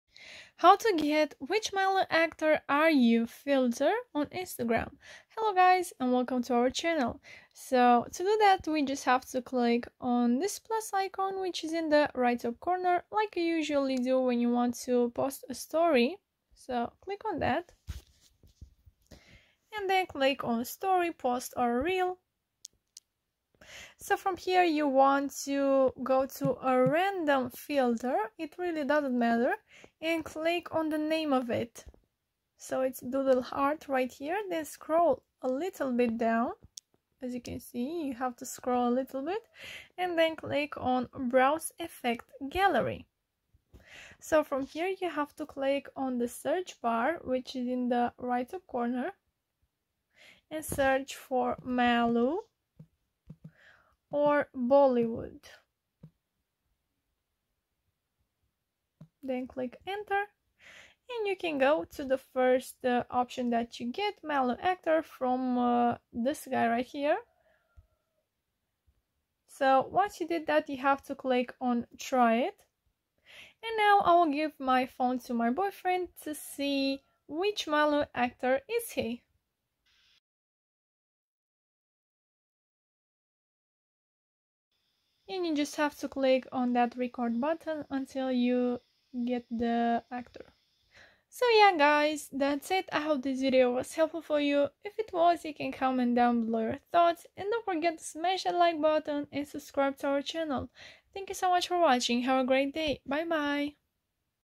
How to Get Which Mallu Actor Are You Filter on Instagram
How to Get Which Bollywood Actor Are You Filter on Instagram

Working 100%! Super Easy tutorial!!! In this video I'm showing you How to Get Which Mallu Actor Are You Filter on Instagram !

Also you can check this video on How To Add Multiple Photos In One Instagram Story 2021 | Android and Iphone: